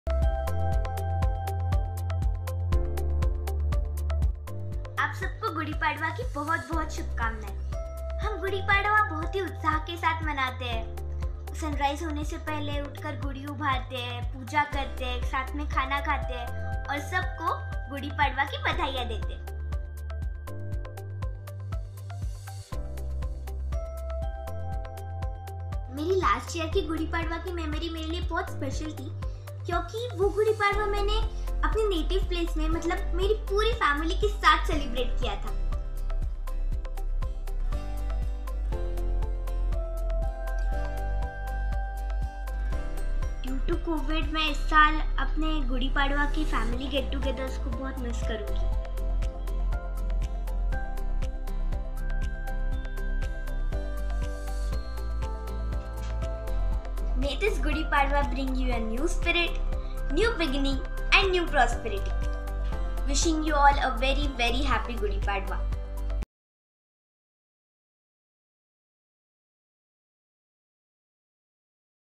Exclusive: Gudi Padwa special with Aditi Jaltare | Punyashlok Ahilyabai
Sony TV's PunyashlokAhilyabai actress AditiJaltare talks about the festival